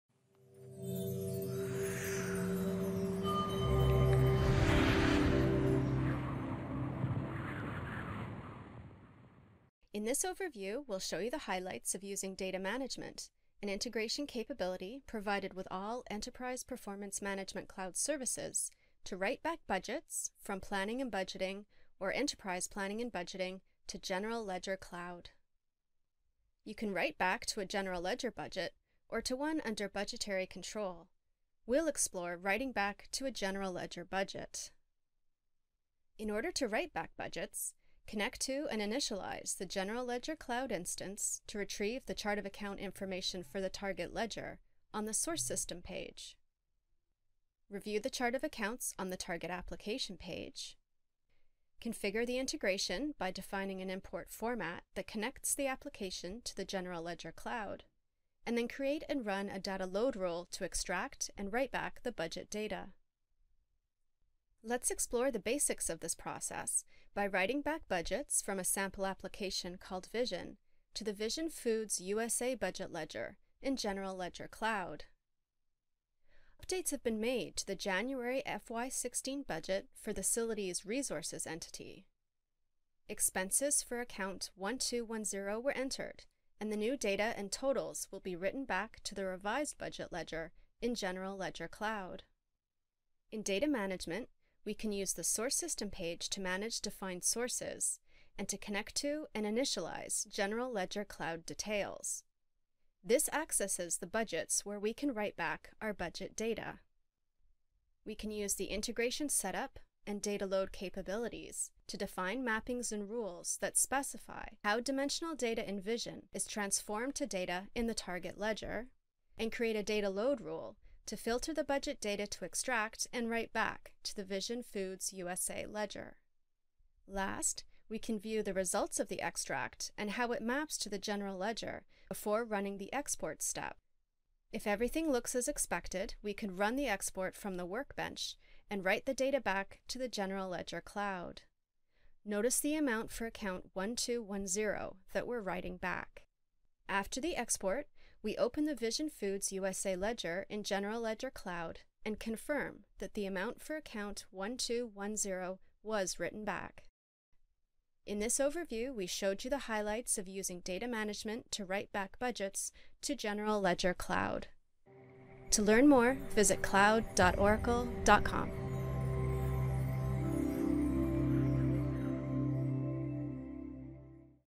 Overview: Writing Back Enterprise Performance Management Budgets to Oracle General Ledger Cloud
This overview describes how you can write back Enterprise Planning and Budgeting and Planning and Budgeting budget data to General Ledger Cloud.